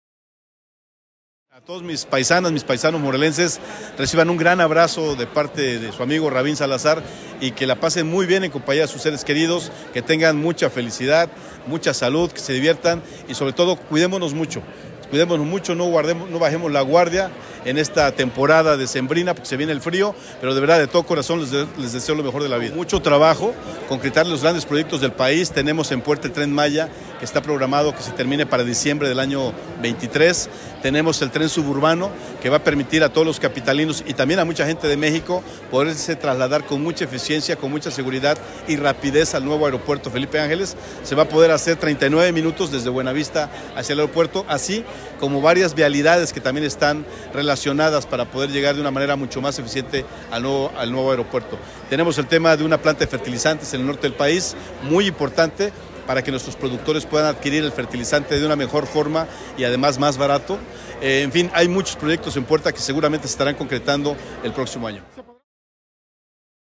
En entrevista con Rabindranath Salazar Solorio.
En entrevista con Rabindranath Salazar Solorio, coordinador general de Política y Gobierno de la Presidencia de la República.